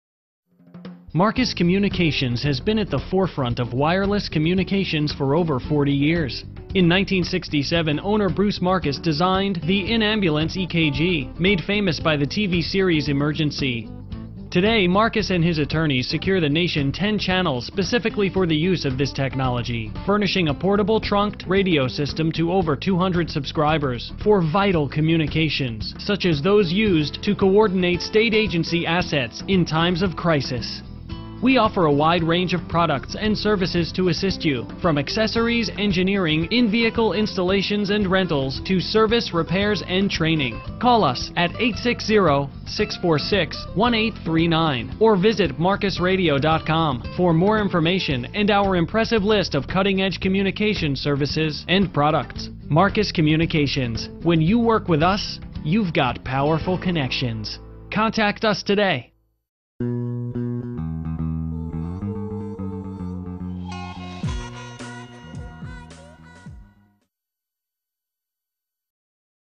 Marcus Communications & Electronics | Manchester, CT
Be able to communicate ALWAYS with Marcus Communications & Electronics. Based in Manchester, Connecticut, and founded by the creator of in-ambulance EKGs (made famous by the TV series "Emergency"), they offer vital communications in times of emergency and ordinary times. Providing in-vehicle installations, satellite communications, two-way radios, tower sites, handheld radios and other systems, they keep everyone tuned in and talking.